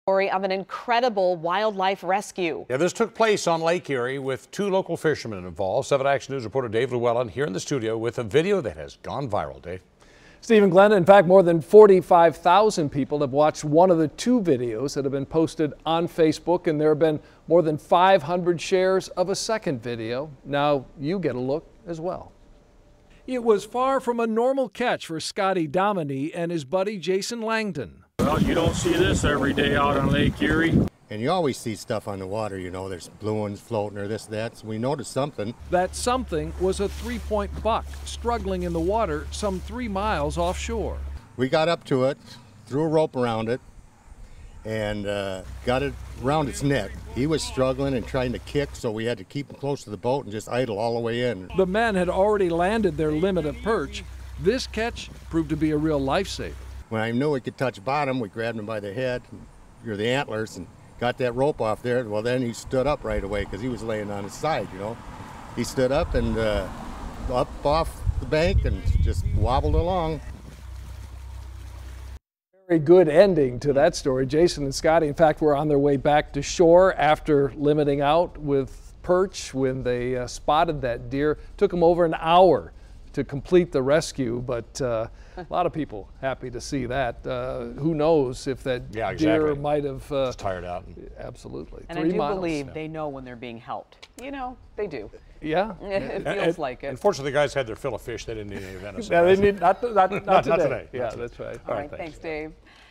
Video of incredible wildlife rescue in Lake Erie goes viral
Video of the incredible rescue of a deer on Lake Erie is going viral on the internet.